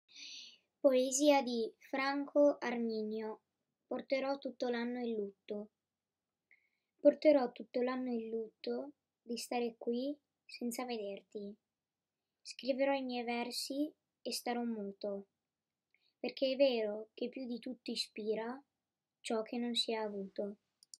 Porterò tutto l’anno in lutto di Franco Arminio
Porterò tutto l’anno il lutto
di stare qui senza vederti.
Scriverò i miei versi e starò muto
perché è vero che più di tutto ispira
ciò che non si è avuto.

Franco Arminio